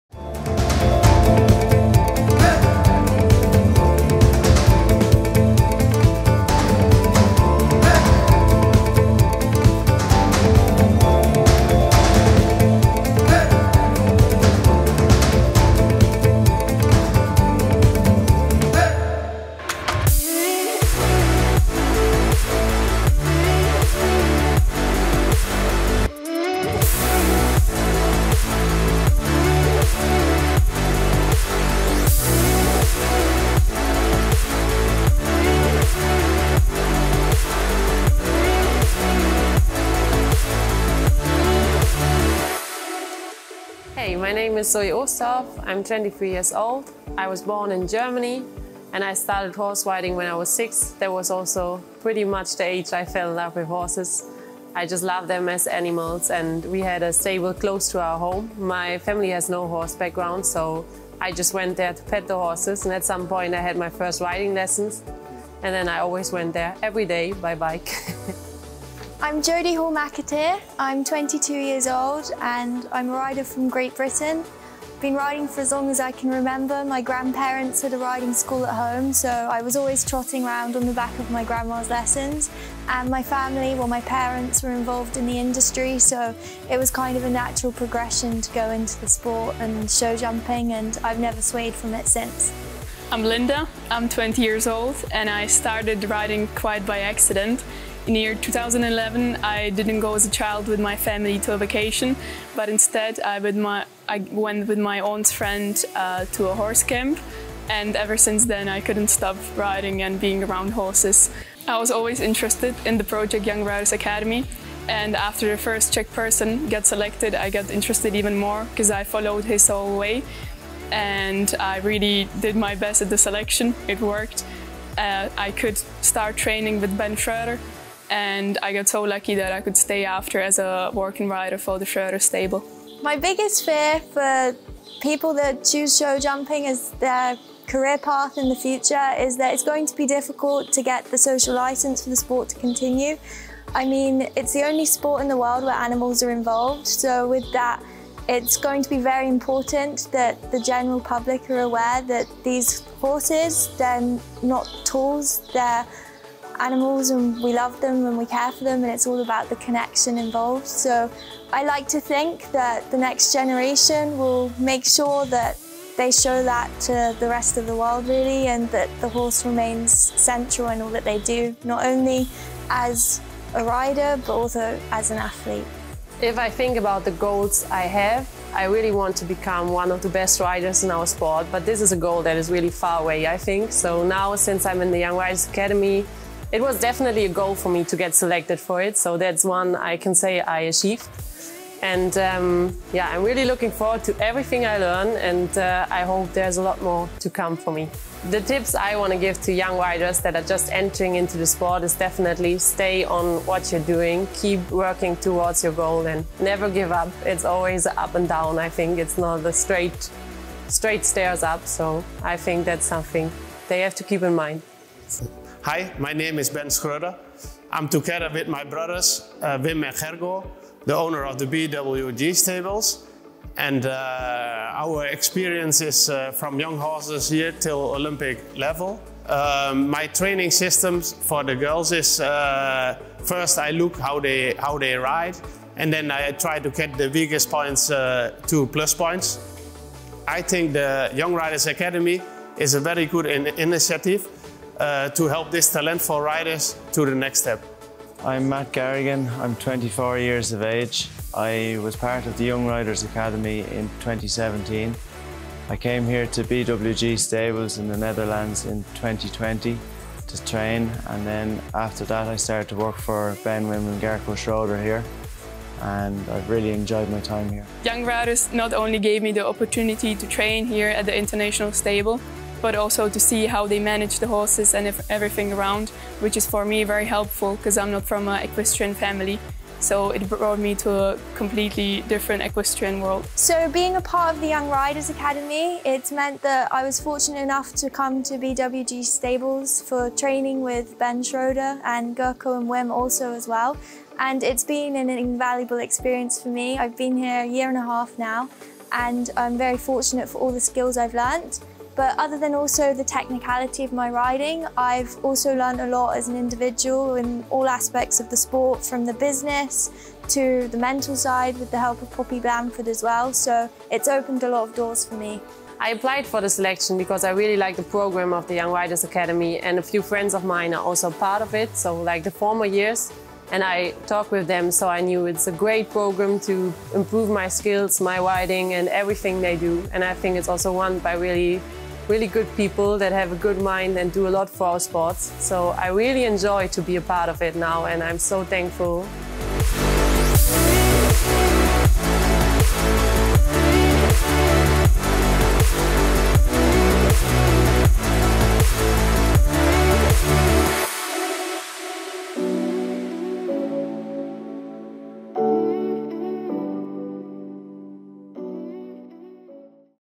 BEHIND THE RIDER -YRA documentary series - 2nd episode
Zoe, Jodie, Linda and Matt at BWG stable with Ben Schroder as trainer